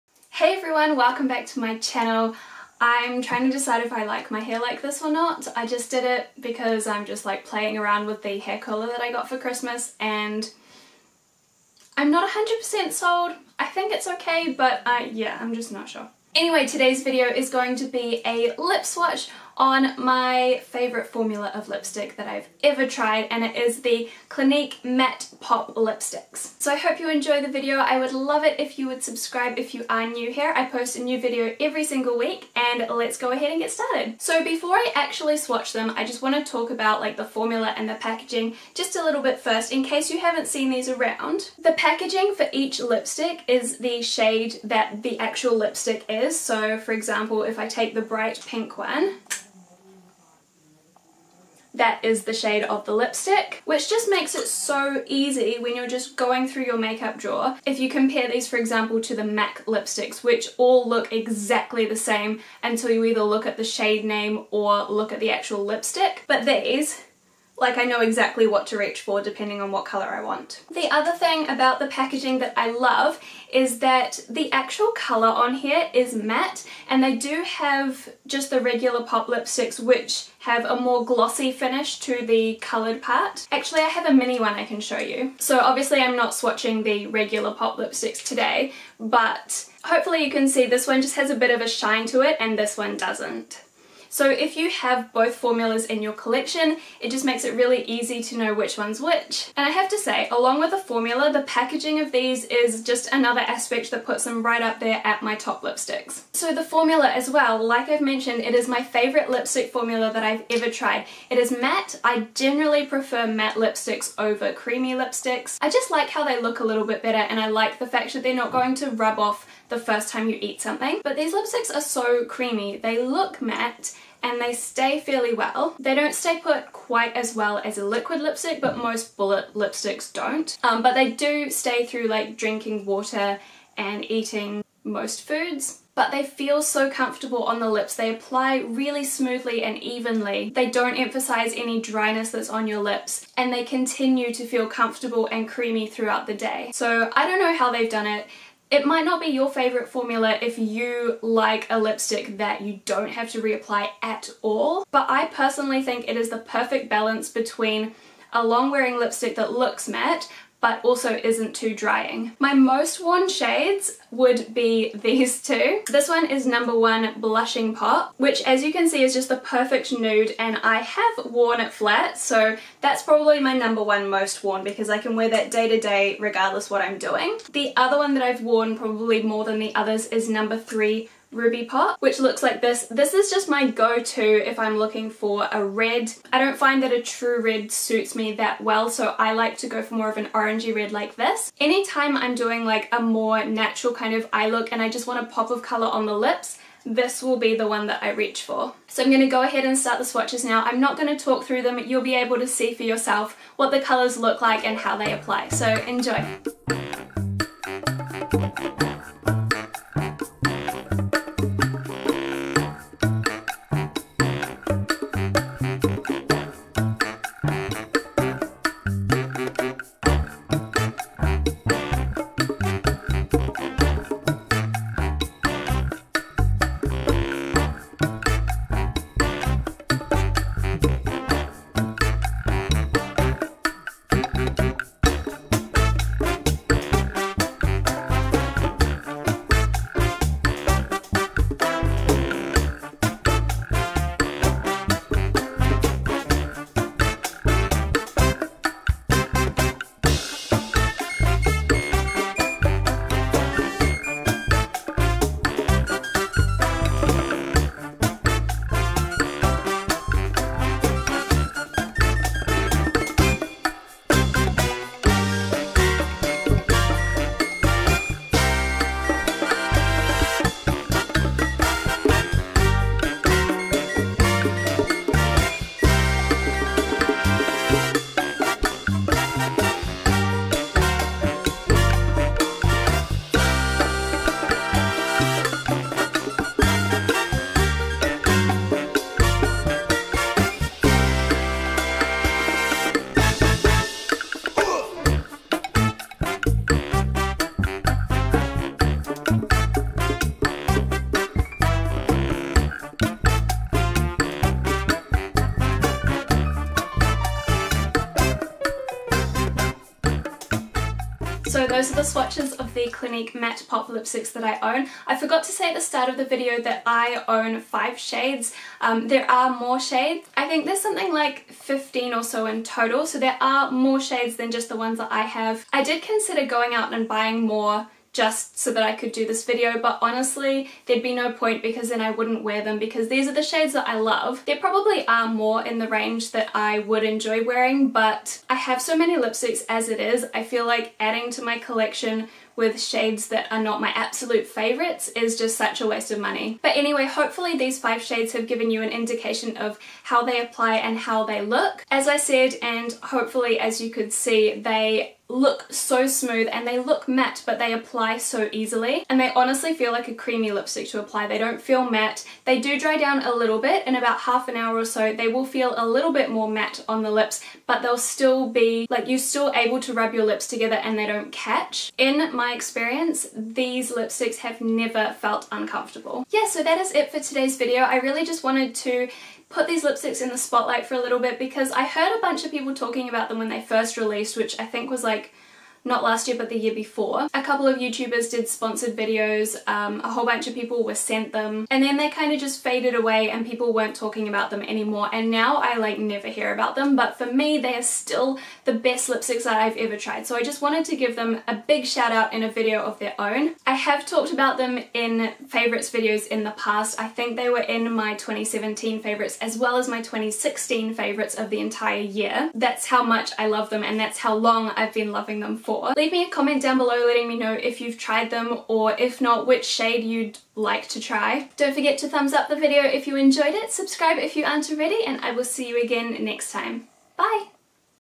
CLINIQUE MATTE POP LIPSTICK SWATCHES | BONBONZZ
Hey guys, today's video is a swatch video of the Clinique Matte Pop lipsticks that I own. I have 5, but there are 16 in total.
01-Blushing Pop
02-Icon Pop
03-Ruby Pop
05-Graffiti Pop
07-Pow Pop
Have you tried this formula? It still holds my #1 favourite lipstick formula I have tried!

What I use:
Camera: Panasonic Lumix DMC-G7
Ring Light: NG-65C Ring Light
Box Light: Sun 400 Fluorescent Lighting Kit
Editing: Sony Vegas Movie Studio Platinum 13.0